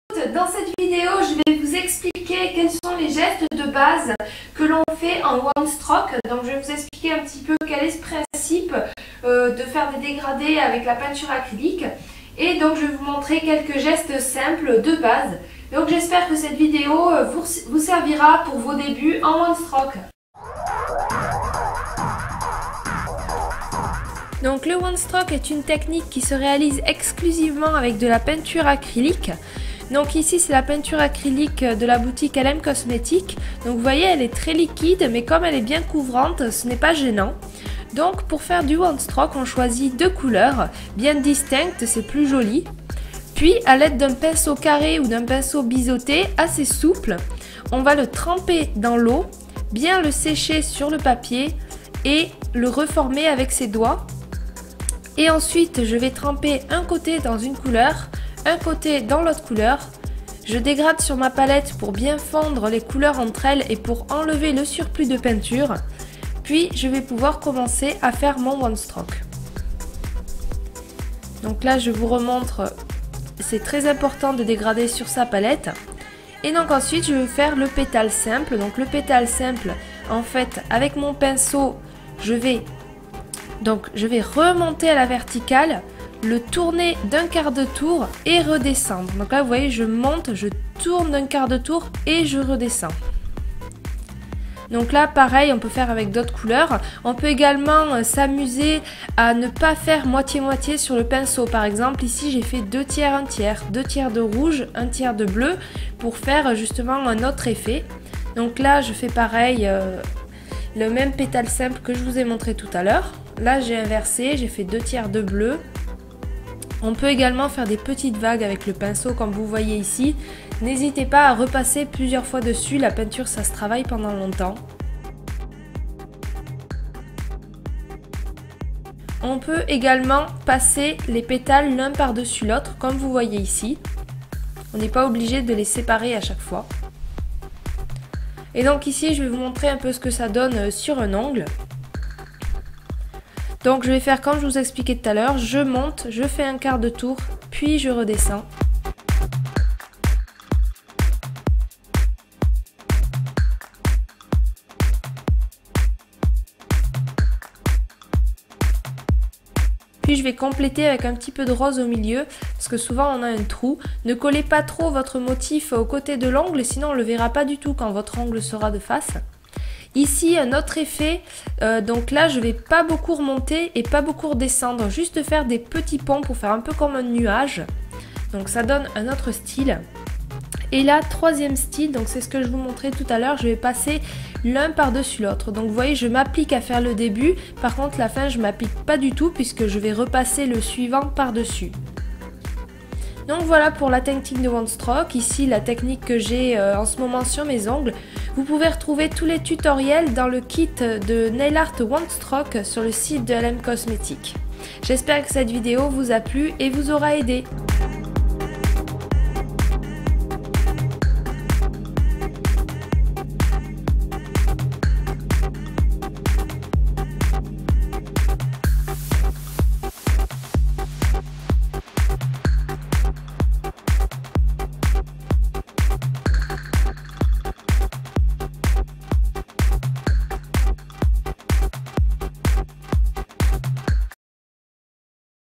Tutoriel : débutants en one stroke, apprendre le bon geste nail art
Vous débutez et vous voulez faire du one stroke ? Cette vidéo vous montre la base du one stroke pour faire de beaux pétales.

Matériel utilisé :
Peintures acryliques LM COSMETIC
Pinceau one stroke souple LM COSMETIC

Distribution Wizdeo.